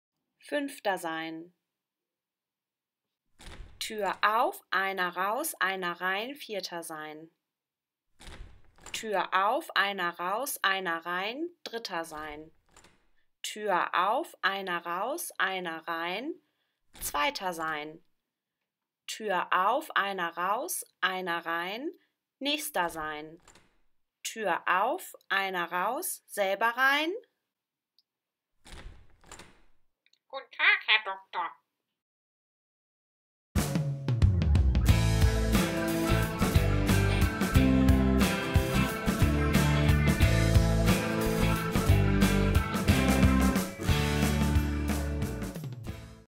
Fünfter Sein
Referendar*innen aus den Bereichen Deutsch und Bildungswissenschaften haben in den Makemedia Studios Trickfilme zu verschiedenen Gedichten erstellt.